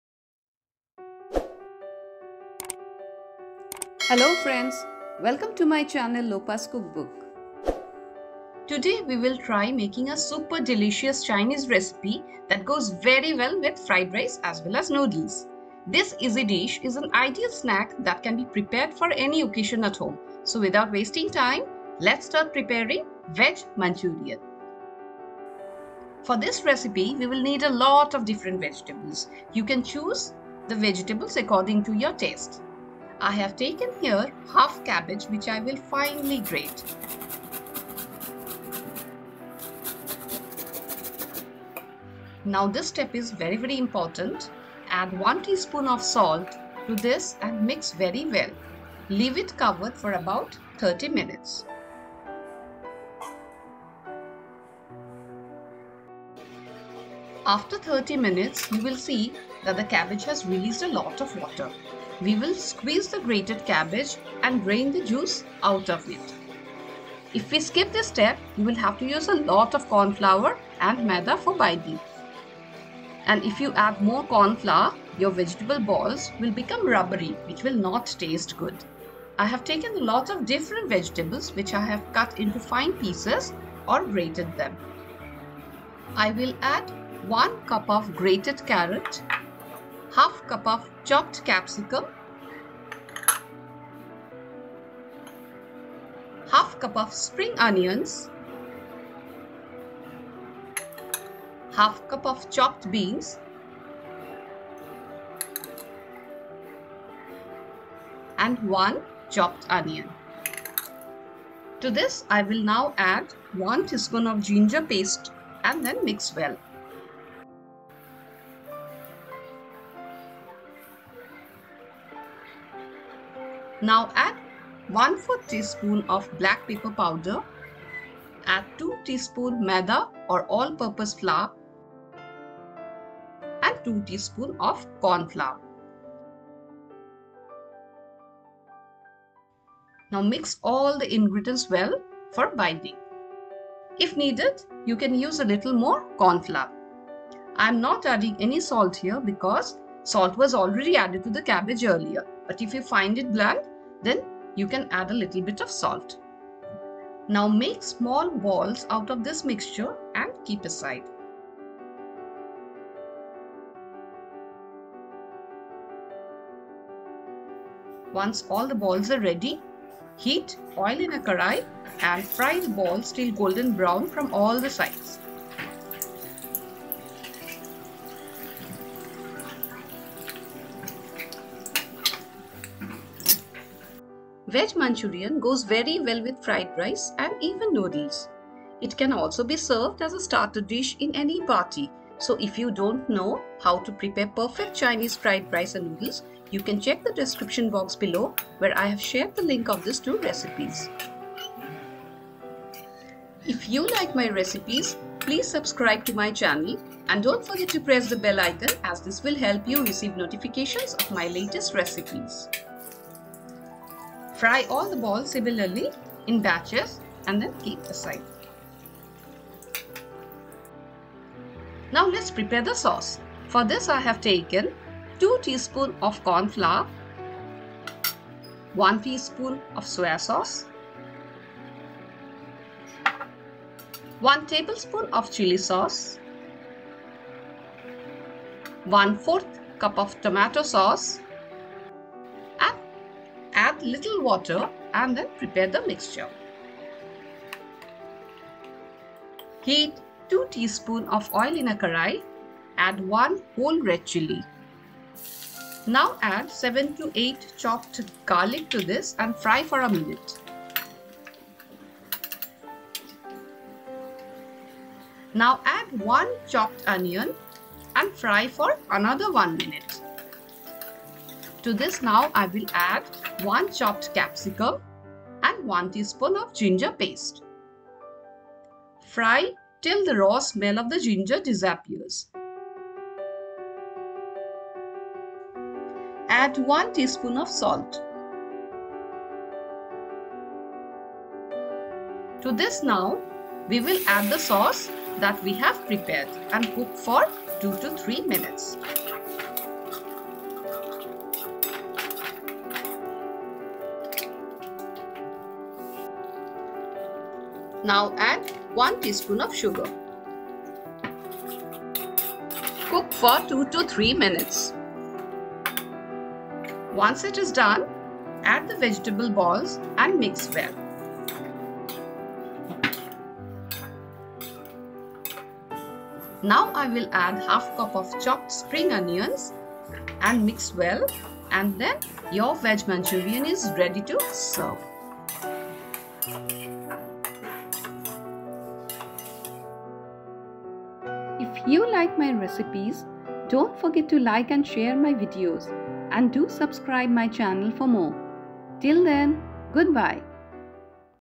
How to make Veg Manchurian/ Veg Manchurian dry recipe/ Make restaurant style veg manchurian/
Today we will try making a super delicious chinese recipe Veg Manchurian.

1. Cabbage:  1/2
2. Carrot: 1
3. Capsicum: 1
4. Spring onions: 1 ½ cup
5. Onions: 2
6. Garlic: 7-8 cloves
7. Ginger paste: 2tsp
8. Soya sauce: 1 tsp
9. Chilli sauce: 1 tbsp
10. Tomato sauce : ¼ cup
11. Corn flour:  4 tsp
12. Maida: 2 tsp
13. Black pepper powder:  ¼ tsp
14. Salt:  as per taste
15. Sugar: 1 tsp
16. Dry  whole red chilli: 1
17. Oil for frying

How to prepare:

• I have taken here half cabbage which I will finely grate.
• Add 1 tsp of salt to this and mix very well. Leave it for about 30 mins.
• After 30 mins we will squeeze the grated cabbage and drain the juice out of it.
• If we skip this step, you will have to use a lot of corn flour and maida for binding. And if you add more corn flour, your vegetable balls will become rubbery.
• Add 1 cup of grated carrot, ½ cup of copped capsicum,  ½ cup of spring onions, ½ cup of chopped beans and 1 chopped onion.
• Add 1 tsp of ginger paste to the vegetables, and mix well.
• Now add ¼ tsp of black pepper powder, add 2 tsp maida or all purpose flour and 2 tsp of corn flour. Mix all the ingredients well for binding.
• If needed you can use a little more corn flour.
• I am not adding any salt here, because salt was already added to the cabbage earlier, but if you find it bland then add a little salt.
•  Now make small balls out of the mixture and keep aside.
• Heat oil in a Karai and fry the balls till golden brown from all sides.
• Fry all the baalls similarly in batches and keep aside.
• Now lets prepare the sauce. For this I have taken 2 tsp of corn flour, 1tsp of soya sauce, 1 tbsp of chilli sauce,  ¼ cup of tomato sauce. Add little water and prepare the mixture.
• Heat 2 tsp of oil in  a kadhai, add one whole red chilli, 7-8 chopped garlic, fry for a minute and then add 1 chopped onion and fry for a minute.
• Now add the capsicum and 1 tsp of ginger paste. fry till the raw smell disappears. Add 1 tsp of salt
• To this now add the sauce that we have prepared and cook for 2 -3 minutes.
• Add 1 tsp sugar
• Then add the veg balls and mix well.
• Once it is done add ½ cup of chopped spring onions and your veg manchurian is ready to serve.